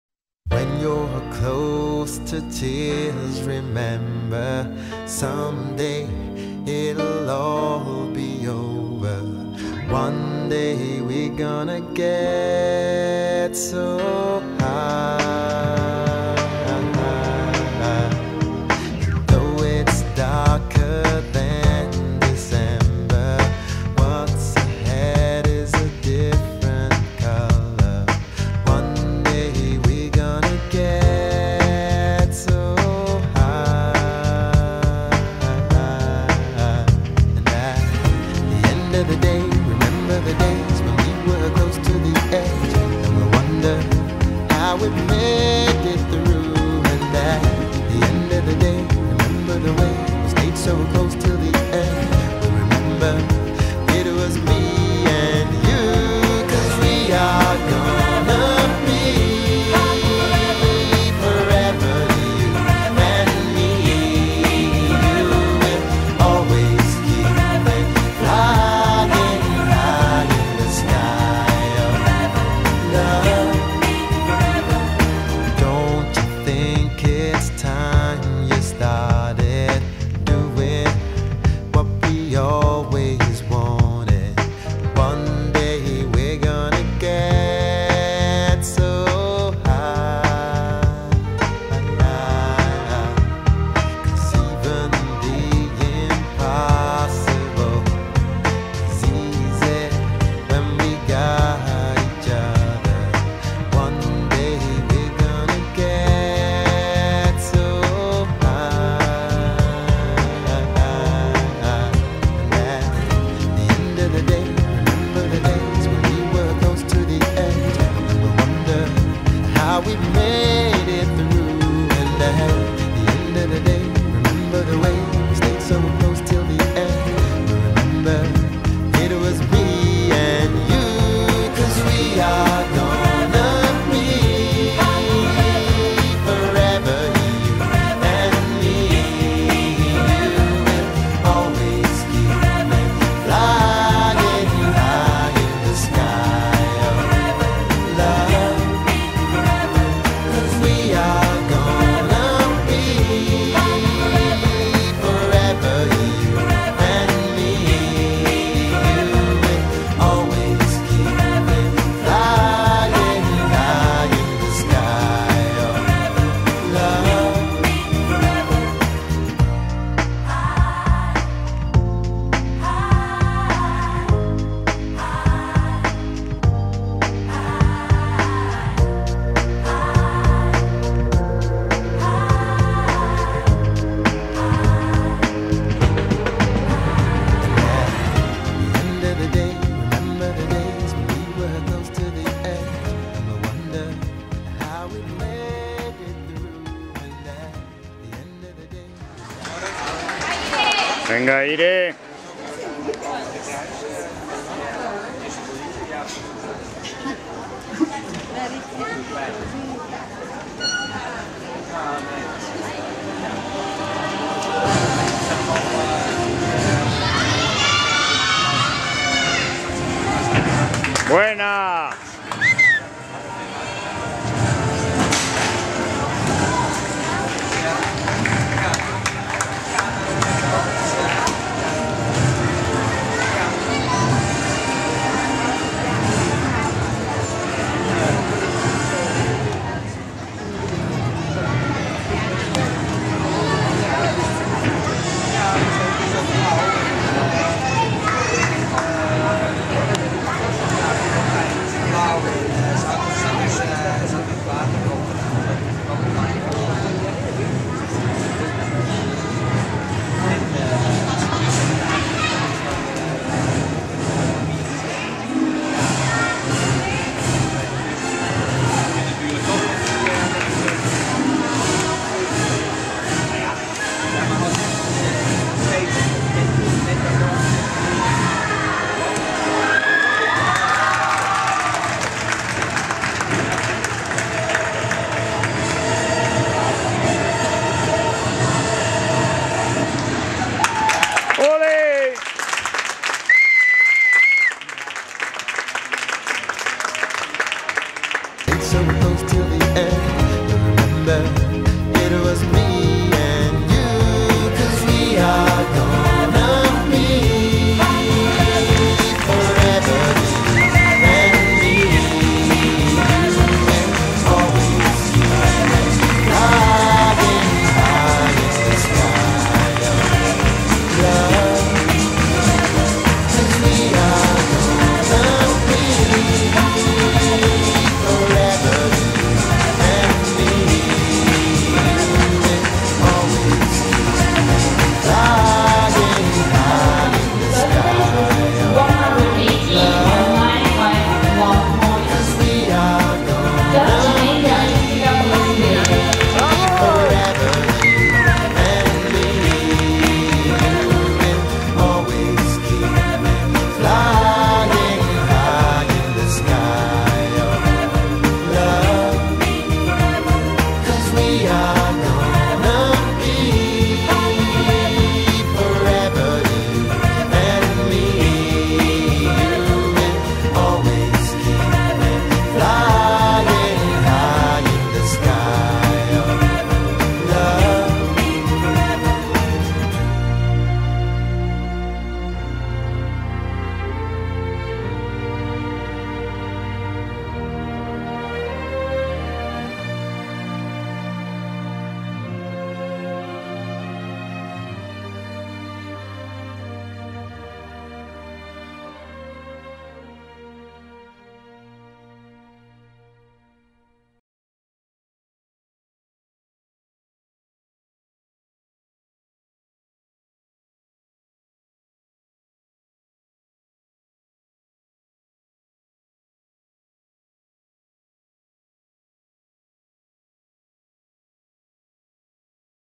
Irene Matienzo Christmas Gym Cup Luxemburgo
Competición Internacional en Bettembourg (Luxemburgo) el pasado 17 de diciembre 2011. Compitieron gimnastas de 9 paises europeos. Irene quedó segunda clasificada detrás de la gimnasta belga y El CGA de Pozuelo acabó tercer clasificado.